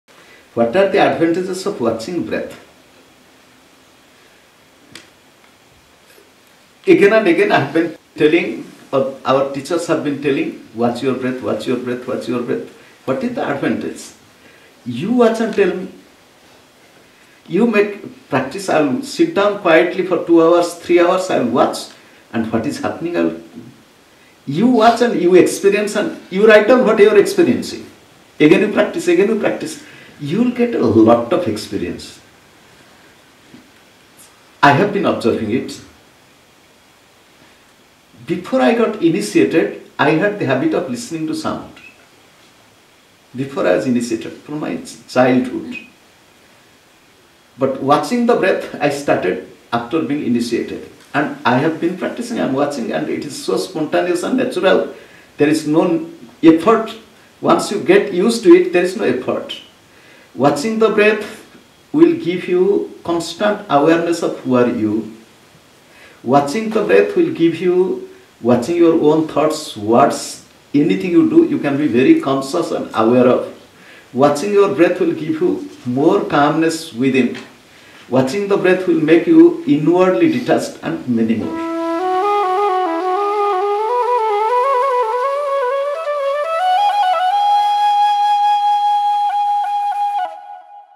What Are the Advantages of Watching Breath?
Paramahamsa Prajnanananda, the reverted Guru of Kriya Yoga, always instructs his disciples and students to constantly watch their breath. How does it help? While on one of his tours to New Zealand in the year 2014, the Guru was asked the same question. Listen to the Guru talk about his own practice and share insights of this most practical aspect of Kriya Yoga.